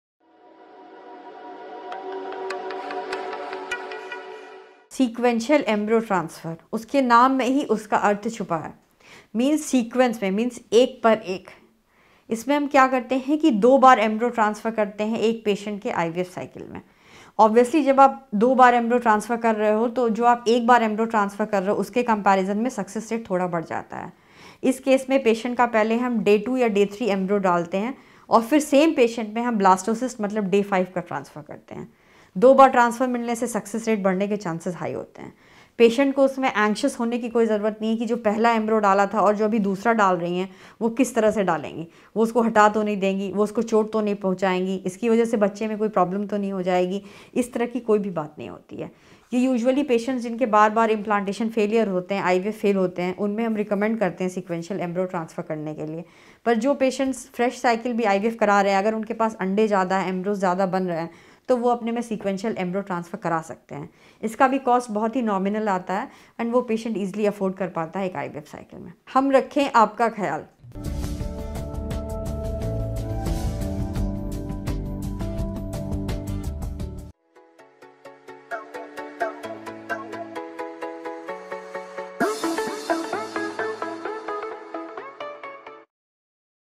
What is Sequential Embryo Transfer? // IVF में क्या है सीक्वेंशियल एंब्रियो ट्रांसफर ?
What is Sequential Embryo Transfer? // IVF में क्या है सीक्वेंशियल एंब्रियो ट्रांसफर ? // Dr. Richika Sahay Shukla,
A policy of sequential or consecutive transfer on Day 3 and Day 5 embryos in patients with adequate number of retrieved oocytes ,yields the advantages of a good embryo implantation rate, satisfactory pregnancy rate and an acceptable multiple pregnancy rate at the same time avoiding complications of blastocyst transfer.